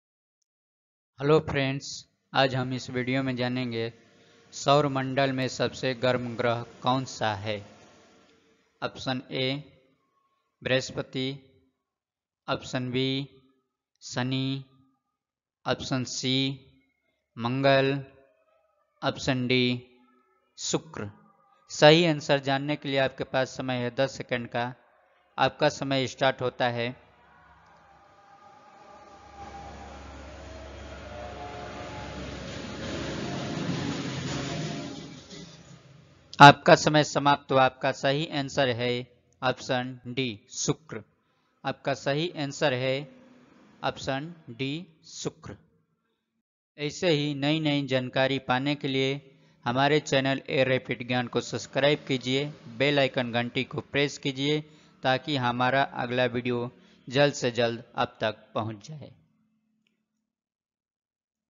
saurmandal me sabse garm grah kaun sa hai ? सौरमंडल में सबसे गर्म ग्रह कौन सा हैं ?
सौर मंडल का सबसे गर्म ग्रह है शुक्र. हालांकि बुध ग्रह सूरज के सबसे क़रीब है इसलिए उसे सबसे गर्म होना चाहिए लेकिन सूरज के इतने क़रीब होने की वजह से ही उसका वायुमंडल नष्ट हो चुका है. जबकि शुक्र का वायुमंडल मुख्यरूप से कॉर्बनडाइऑक्साइड से भरा हुआ है जो एक तरफ़ा रास्ते का काम करता है.